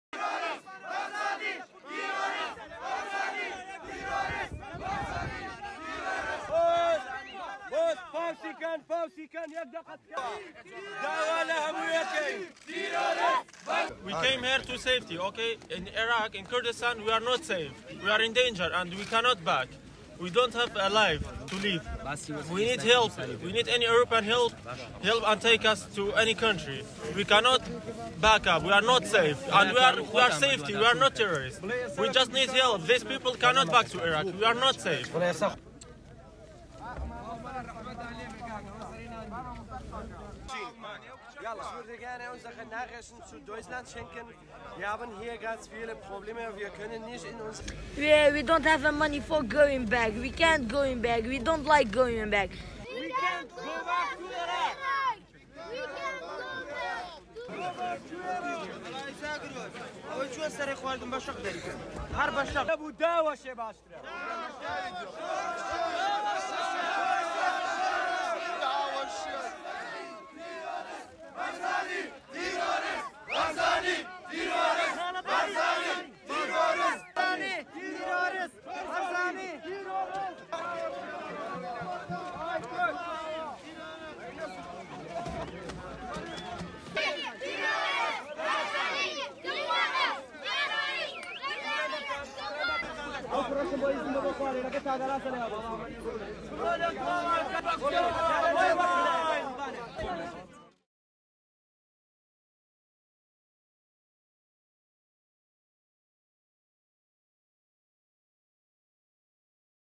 Belarus Poland border migrants protest against being sent back to Iraq - News 360 Tv
Migrants stage mass protest at the Belarus border against being taken back to their home countries.

Crowds chanted 'we can't go back' as banners and signs made pleas for resolution to the crisis at the border between Belarus and Poland after weeks of unrest in the area.

One migrants said: "We came here to safety, okay. In Iraq, in Kudistan, we’re not safe, we’re in danger and we cannot go back, we don’t have a life to live. We need help.

"We need European help, help us, take us to any country. We cannot back up, we’re not safe. And we’re safety, we’re not terrorists. We just need help. These people cannot go back to Iraq, we’re not safe."